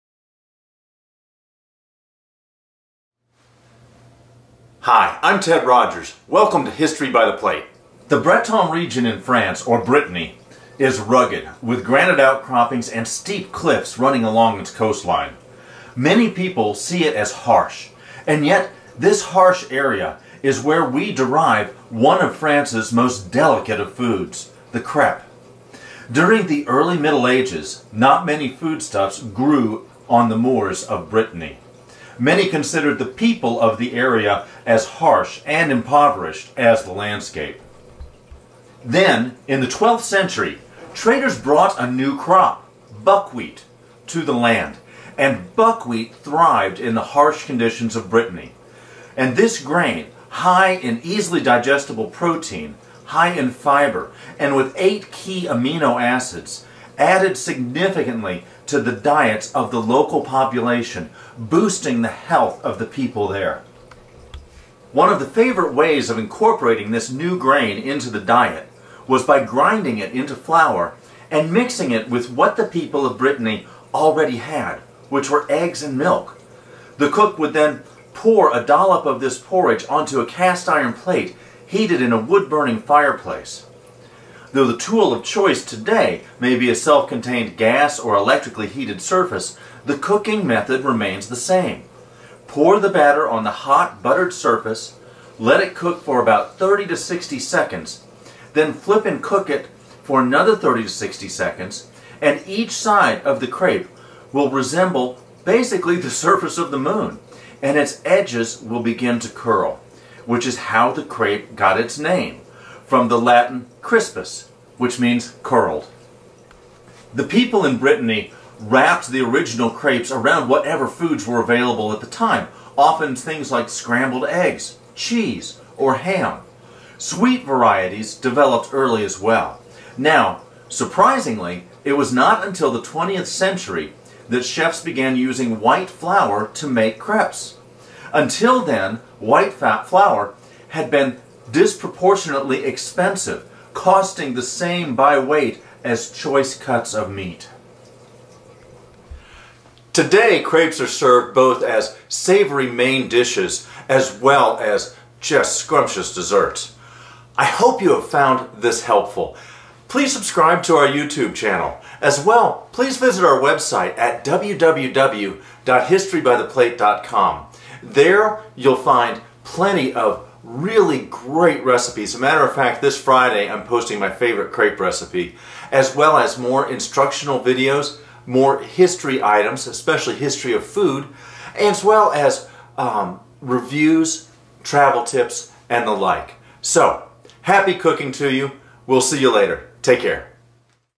The Origins of the Crepe
We here to talk about the unusual beginnings of the French crepe, today. Join us for the history today and come back to visit on Friday for a delicious crepe recipe!